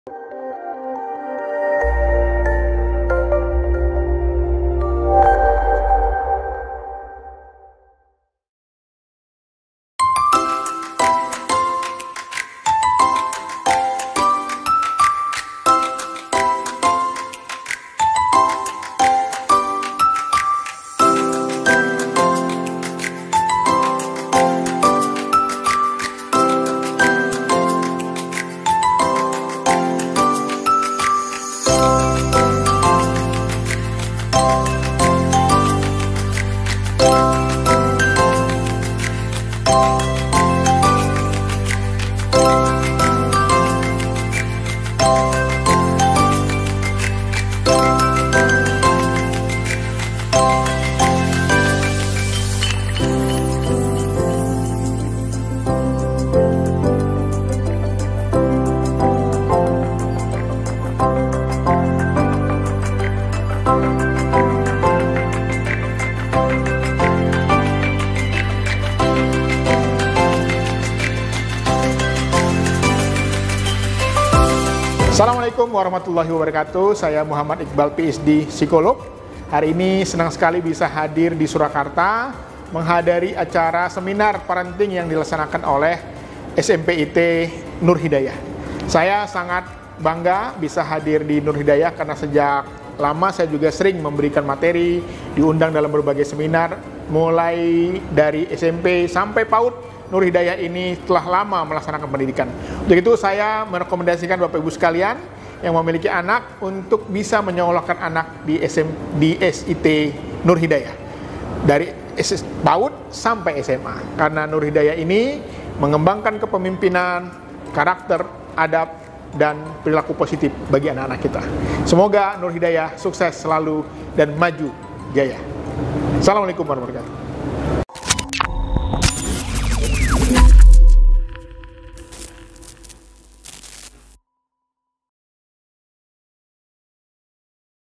Psikolog Iqbal Beri Materi ‘Berdaging’ Pada Seminar Parenting SMPIT Nur Hidayah | Nur Hidayah TV
SMPIT Nur Hidayah selenggarakan Pelantikan Pengurus Komite Periode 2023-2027 dan Seminar Parenting yang diikuti oleh para orang tua siswa pada Sabtu (28/10/2023). Kegiatan ini digelar secara luring di Hotel Swiss Belinn dan daring melalui kanal Youtube SMPIT Nur Hidayah.

Psikolog Iqbal memberikan materi yang sangat berdaging pada seminar parenting yang bertajuk “Bangun Cinta dalam keluarga Raih Bahagia”. Dengan prolog yang ‘menampar’ para peserta seminar. Iqbal memaparkan realita dengan berbalut guyonan.

Pendidikan anak dimulai dari keluarga, jika keluarga harmonis, akur, saling menyayangi tentu berdampak pada mental tangguh seorang anak. Sikap terlalu memanjakan anak pada orang tua saat ini, bisa berdampak pada anak yang menyebabkannya menjadi seorang Strawberry Generation.

Kepala SMPIT Nur Hidayah, Ustadz Bangun Rohmadi S.Pd.I., dalam sambutannya berharap dapat menghadirkan suasana ‘healing’ pada kesempatan tersebut dan menyulut semangat Pendidikan baik di rumah maupun sekolah.

Begitu pun dengan yang disampaikan Ketua Yayasan Nur Hidayah, Ustadz Dr. Kasori Mujahid, M.Ag., yang berpesan agar orang tua dan Yayasan saling bahu membahu dan tolong menolong dalam pendidikan anak. Ustadz Kasori juga menyambut masukan yang membangun dari orang tua siswa.

Sebelum masuk materi, kegiatan dimulai dengan Pelantikan Pengurus Komite Periode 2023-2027 yang diketuai bapak Khoirul Anam. Pelantikan sepuluh pengurus Komite dilakukan oleh Kepala SMPIT Nur Hidayah dan dilanjutkan dengan pemberian ucapan selamat dan foto bersama. [Humas]

Produksi :
Nur Hidayah TV
Bunyikan juga tanda loncengnya agar mendapatkan notifikasi video baru dari NHTV Channel.